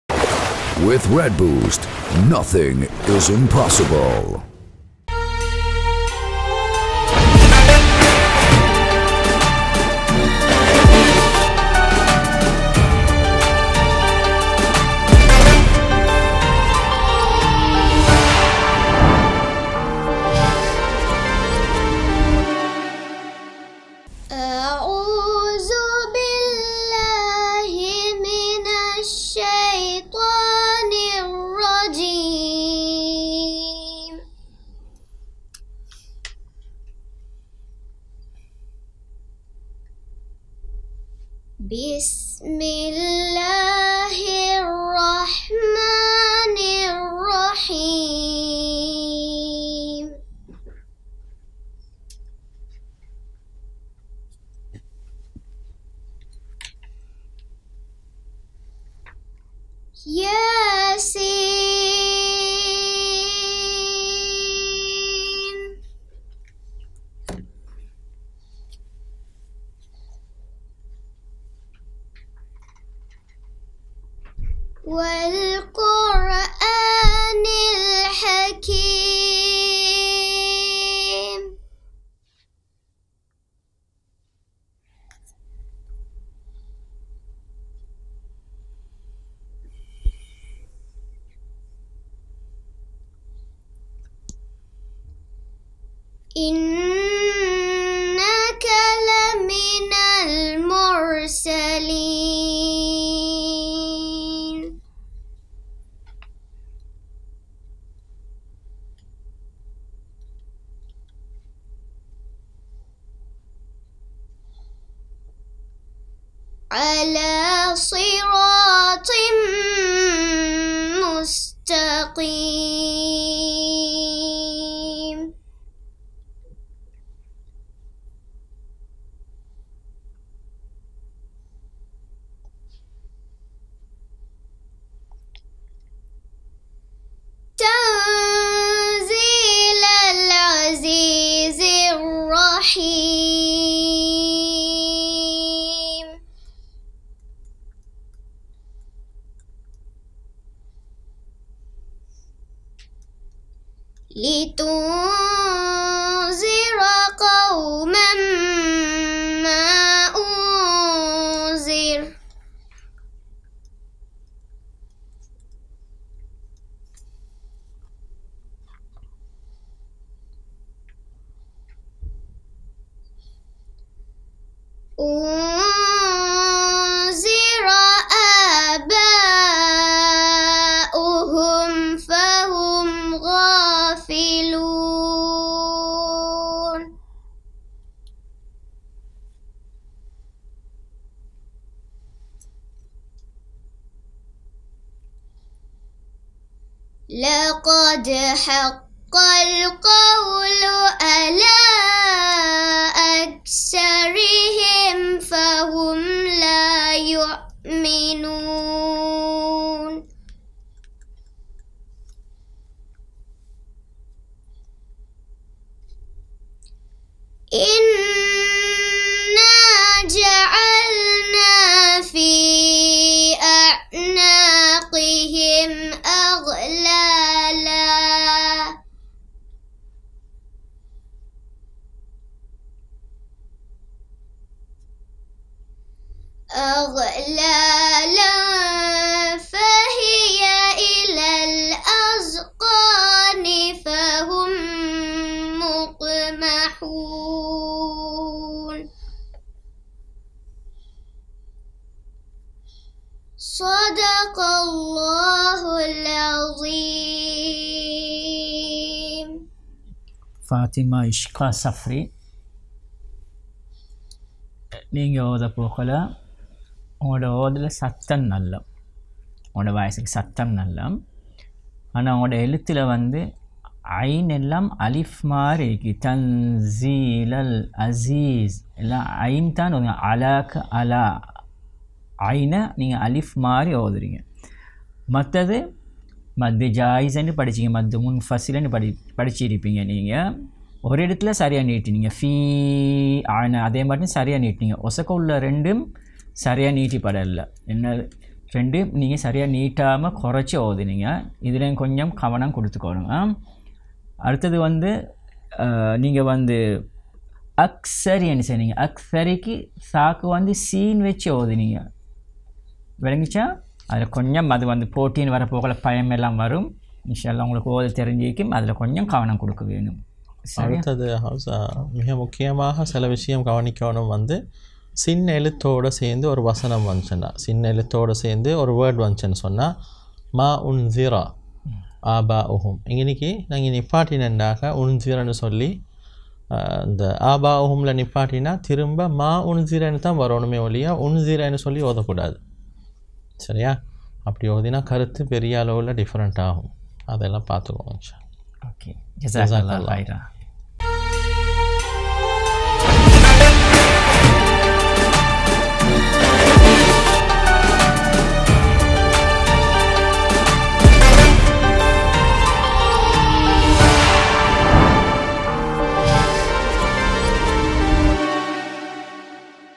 UTV Qirath Competition - 2021 | Fathima Ishka Safry
விந்தைமிக்க குரல்களினால் விந்தை செய்யும் UTV இன் கிராத் முறத்தல் போட்டி - 2021